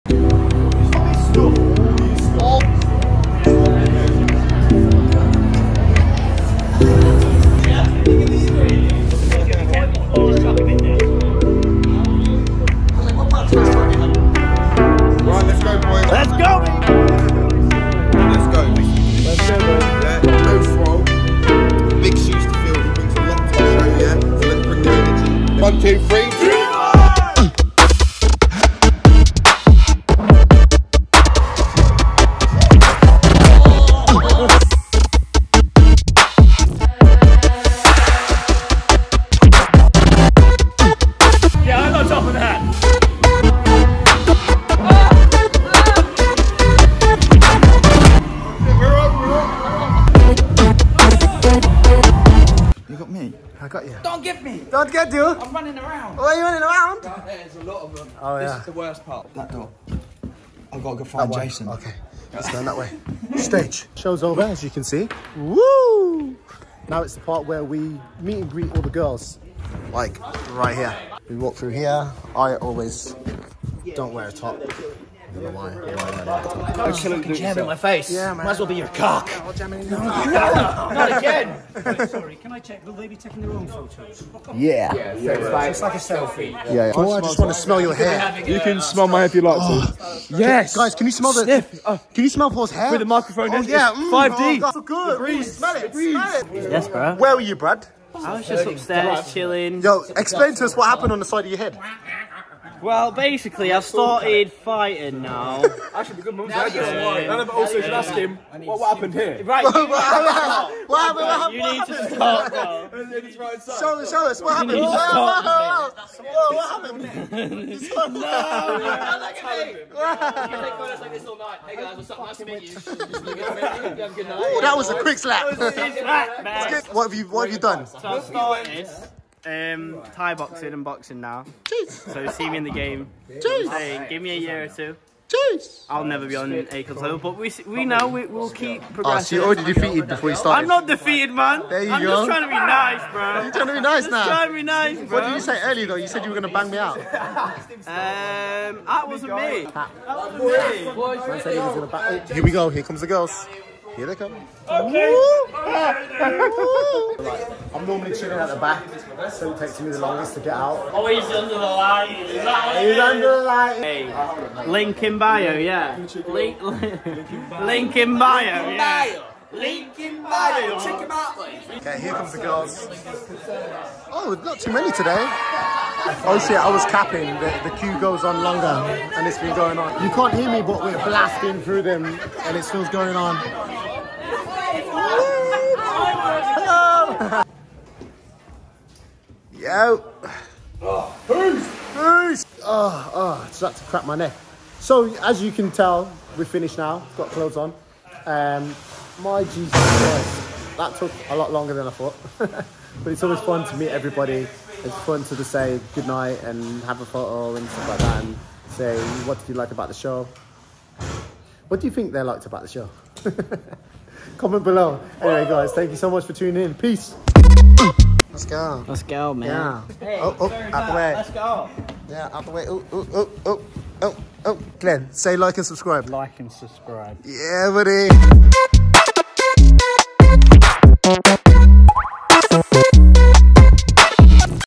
The Dreamboys Tour | Hereford
Just a little snippet of what goes on at the Dreamboys Tour! Its not all just baby oil and press ups... well that happens too but yeah enjoy :)